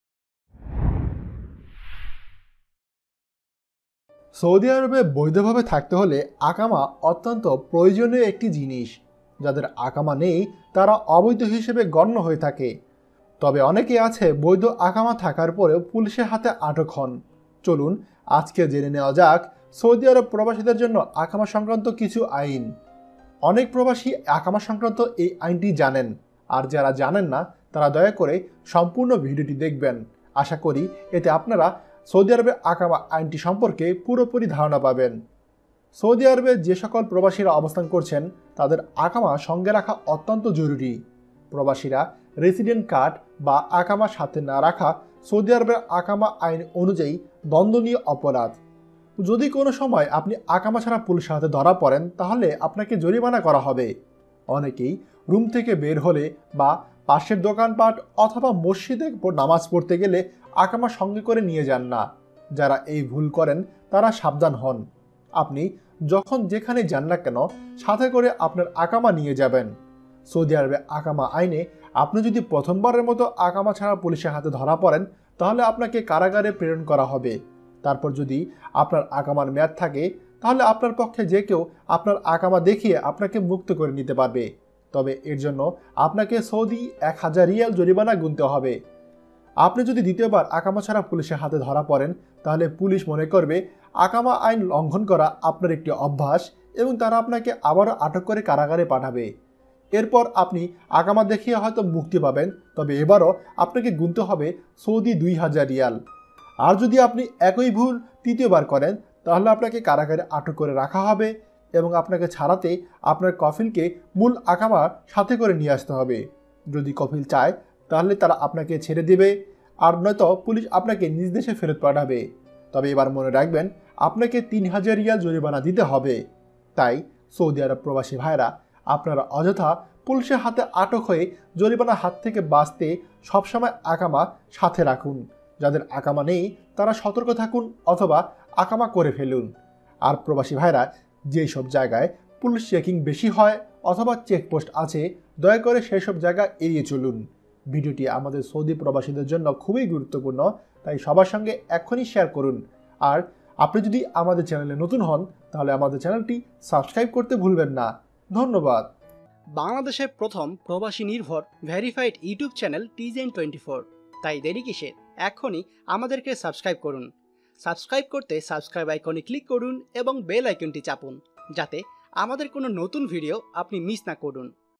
সৌদি আরবের আকামা আইনে কিছুটা পরিবর্তন। বৈধভাবে সৌদি আরবে থাকার জন্য নতুন নিয়মটি জেনে নিন।
সৌদি আরবের আকামা আইনে কিছুটা পরিবর্তন। বৈধভাবে সৌদি আরবে থাকার জন্য নতুন নিয়মটি জেনে নিন। সৌদি আরবের আকামা আইনে কিছুটা পরিবর্তন। বৈধভাবে সৌদি আরবে থাকার জন্য নতুন নিয়মটি জেনে নিন।